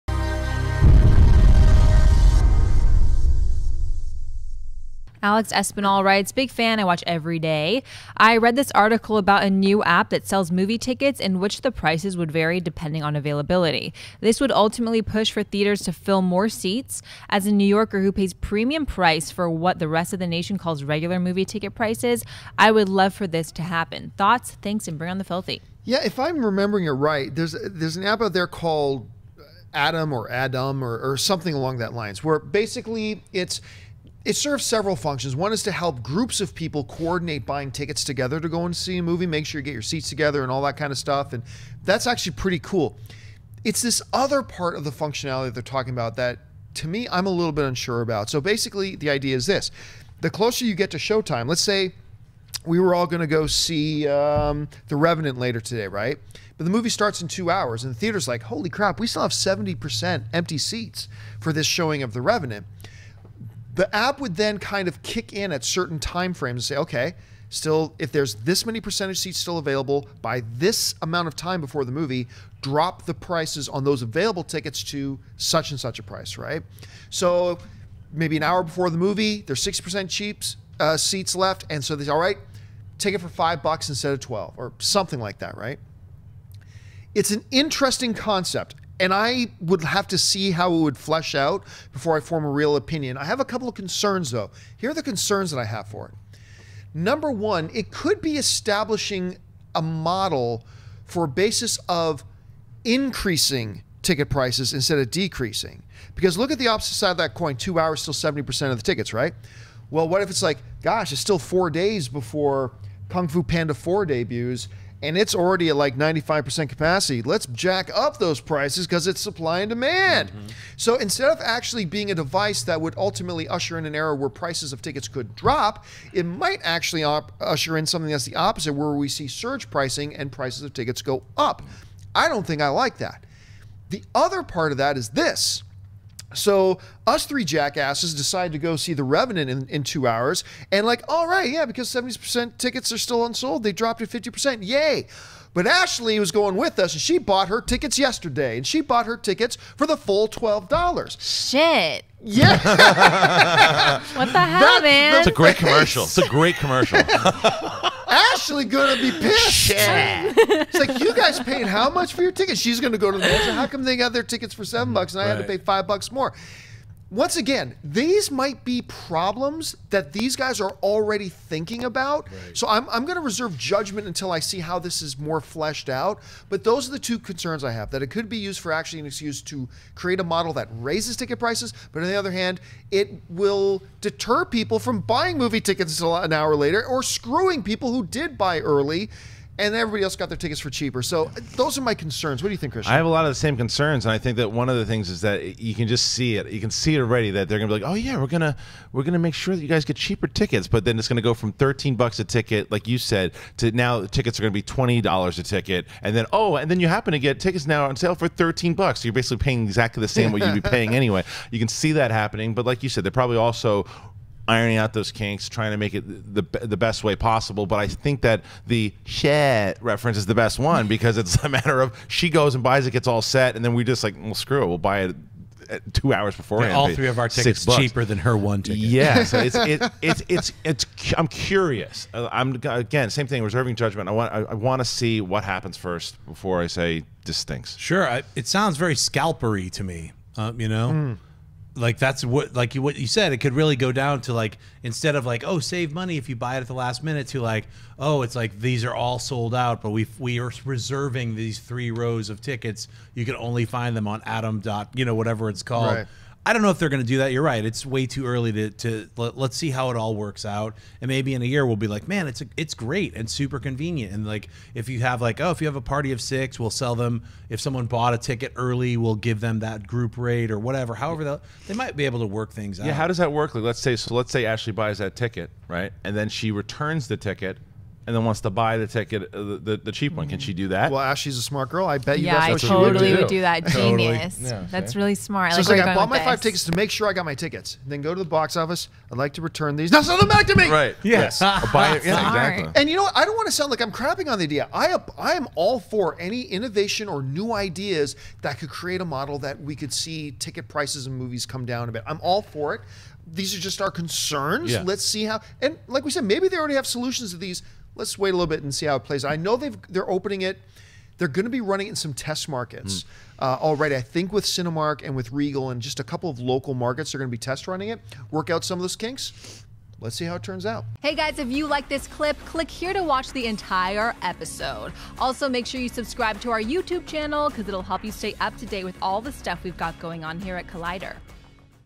New mobile movie ticketing app?- Collider
Alex Espinal writes:

Big fan, I watch everyday! I read this article about a new app that sells movie tickets in which the prices would vary depending on availability. This would ultimately push for theaters to fill more seats. As a New Yorker who pays premium price for what the rest of the nation calls regular movie ticket prices, I would love for this to happen. Thoughts? Thanks and bring on the filthy!